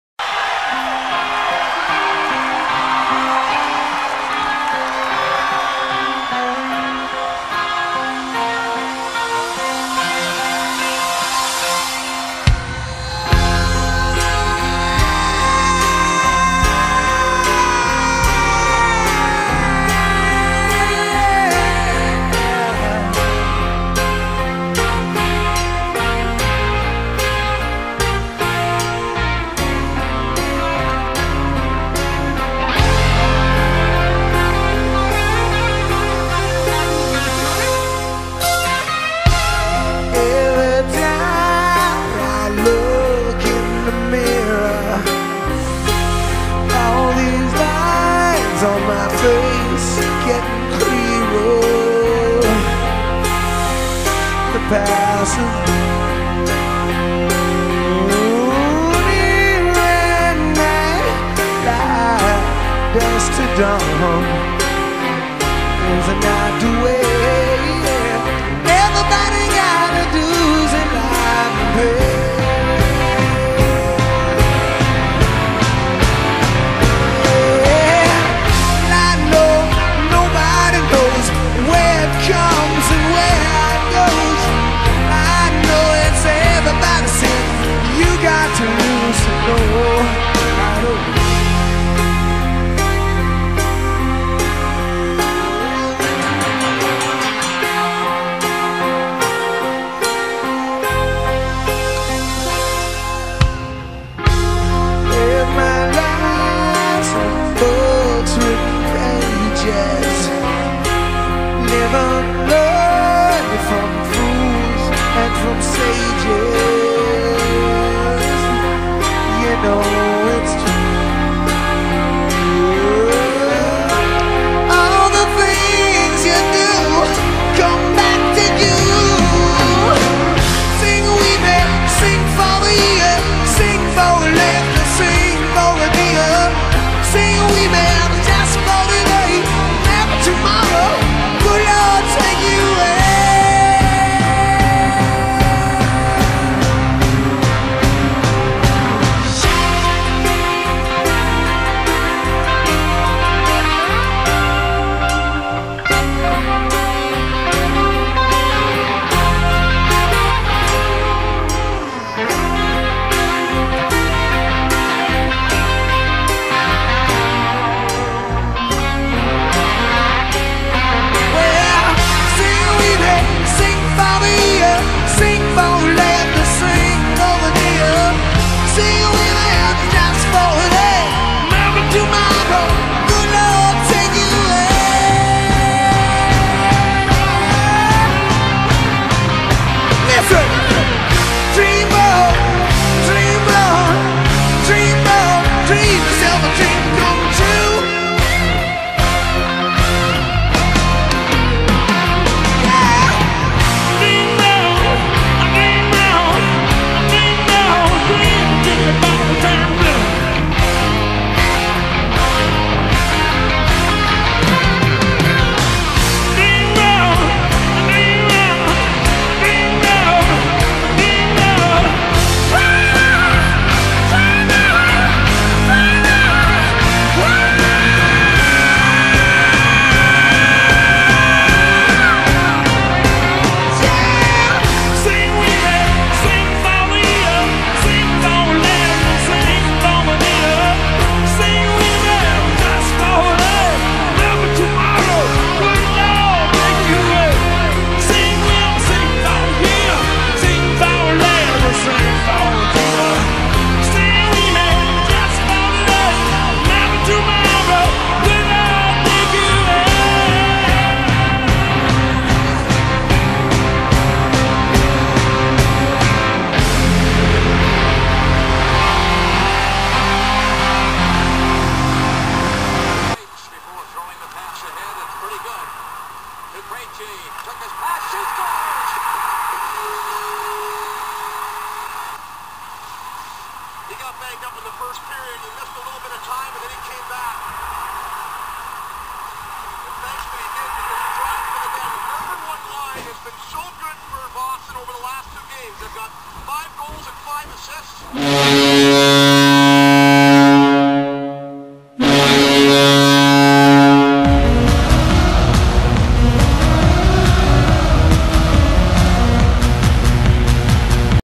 bruins vs. lightning remix
for the first time since 1992, the Bruins are going to the Stanley Cup!